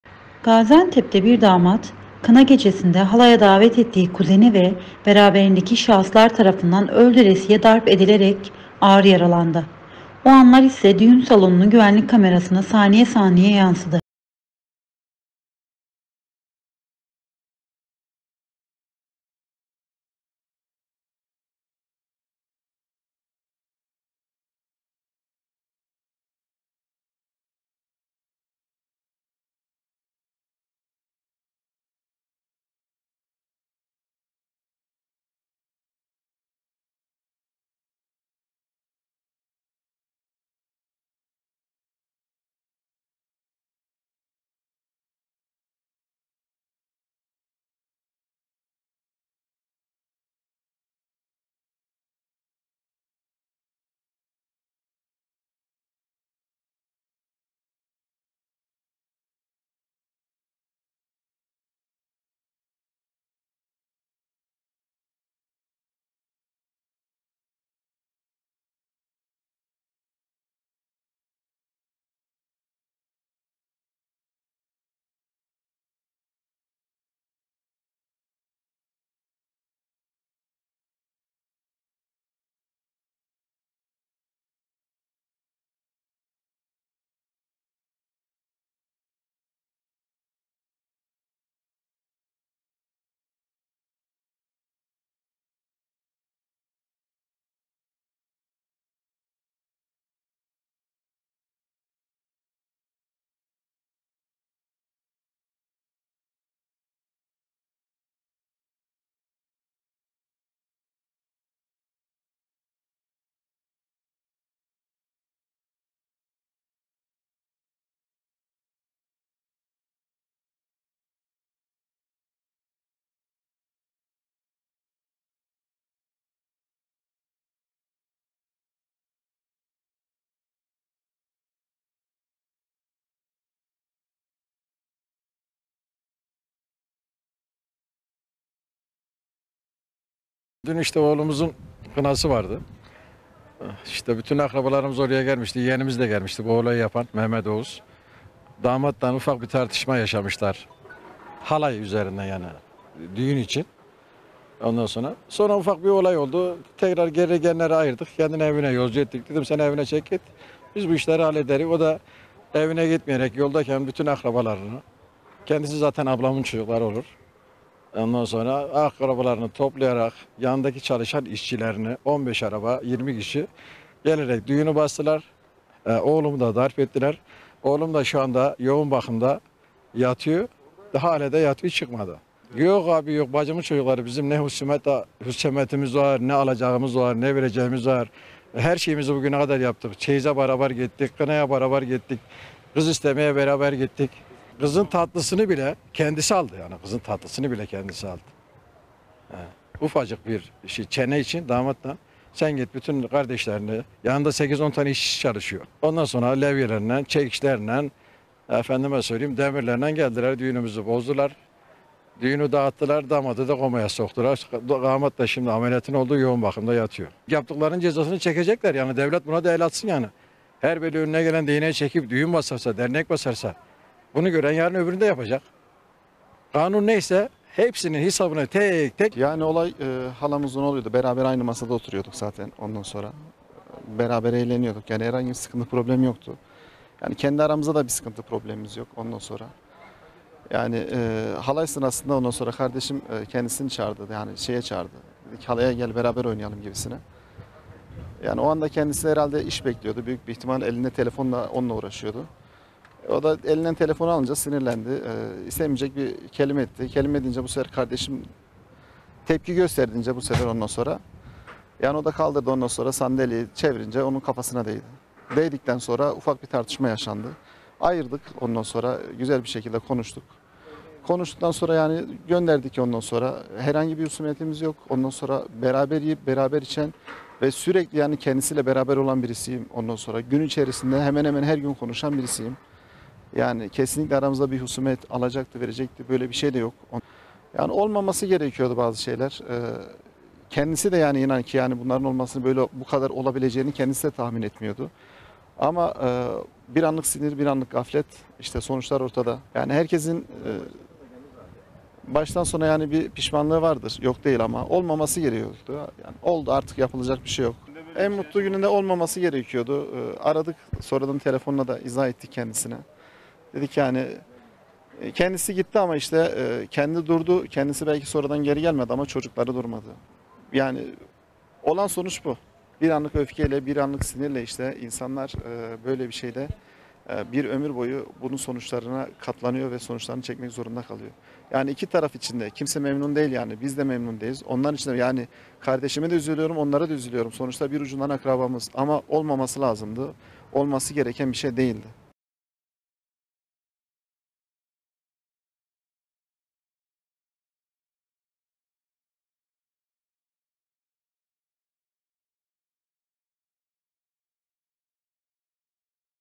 Halay Kavgasında Damat Yaralandı
Kına gecesi kabusa döndü, onlarca kişinin saldırısına uğrayan damat ağır yaralandı